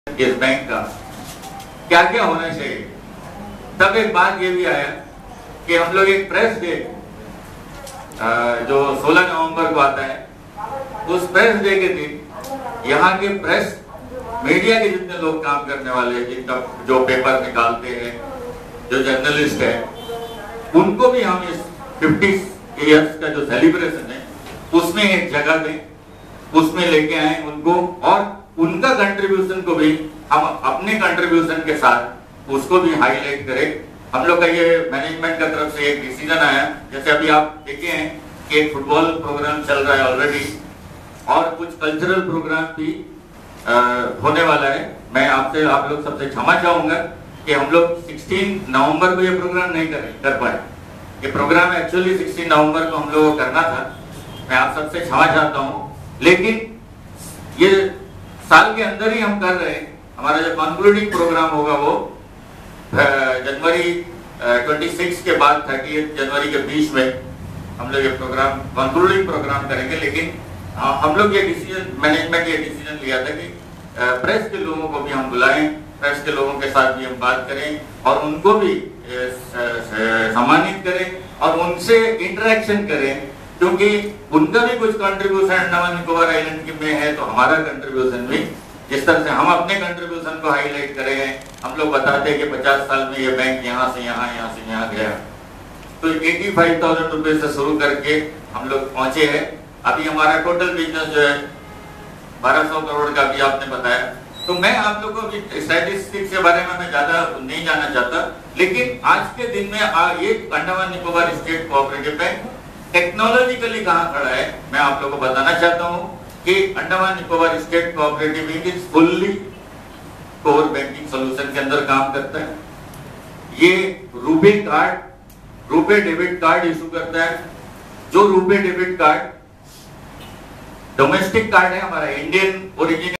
FELICITATION PROG to ALL MEDIA PERSONS BY anscbankEdweepNews12
-FELICITATION PROG- to ALL MEDIA PERSONS BYanscbankEdweepNews (PLEASE EDIT AS U PLEASE)-FELICITATION PROGRAMME ON NATIONAL PRESS DAY to ALL THE MEDIA PERSONS ADDRESS BY CHAIRMAN Andaman and Nicobar State Co-OPerative Bank this forenoon - thanking & thanking ALL !!! (DIGITAL INDIA - DIGITAL DOCUMENTATION under VALUE EDUCATION)- thanking !!!